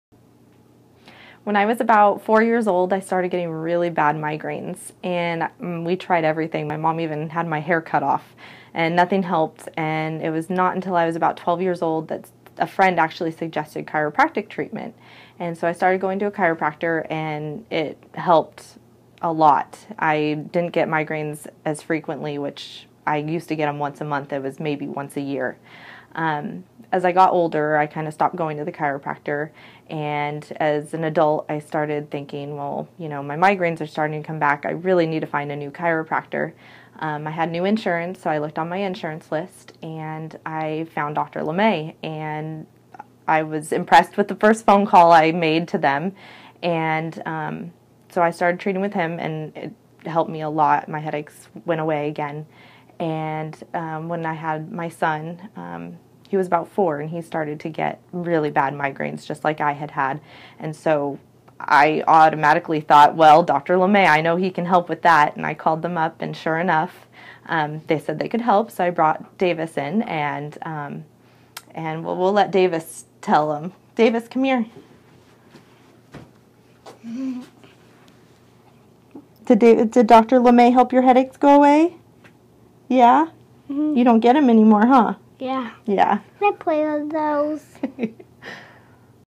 Courtney's chiropractic testimony
Courtney shares her experience when she was little suffering from migraine headaches and how chiropractic helped then and now. And her son, Davis, gets to say a few words, too!